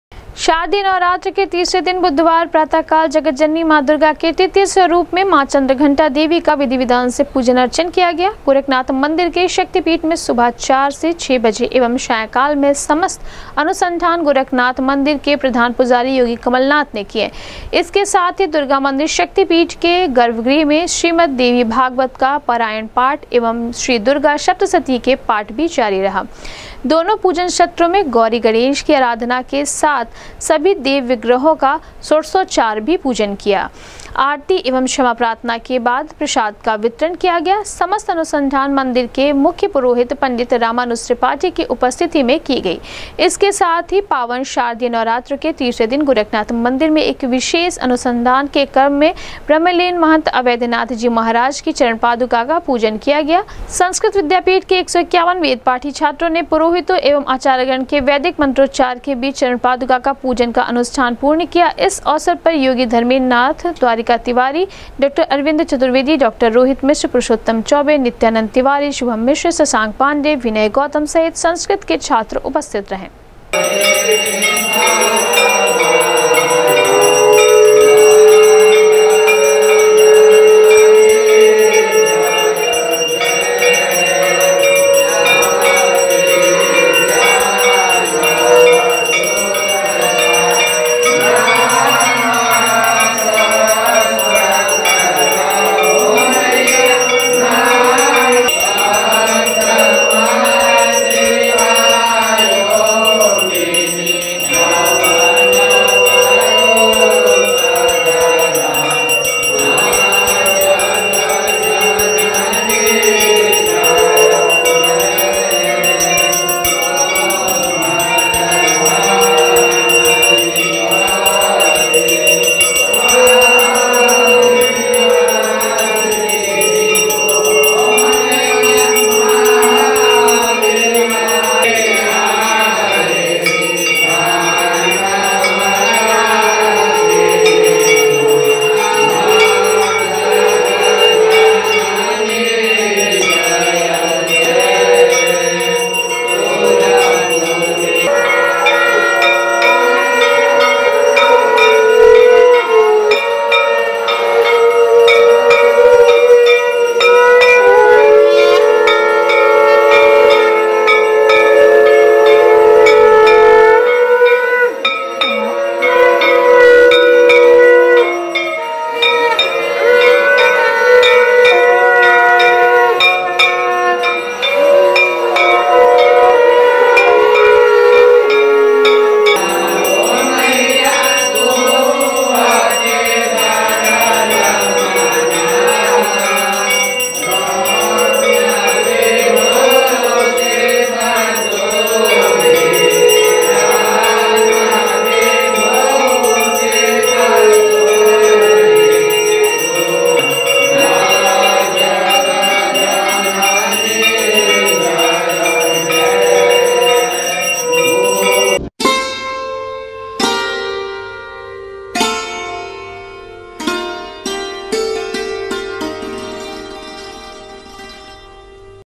शारदीय नवरात्र के तीसरे दिन बुधवार को जगतजननी मां दुर्गा के तृतीय स्वरूप मां चंद्रघंटा देवी का
शारदीय नवरात्र के तीसरे दिन बुधवार को जगतजननी मां दुर्गा के तृतीय स्वरूप मां चंद्रघंटा देवी का गोरखनाथ मंदिर के शक्तिपीठ में विधि विधान से पूजन अर्चन हुआ सम्पन्न, आरती एवं क्षमा प्रार्थना के बाद वितरित किया गया प्रसाद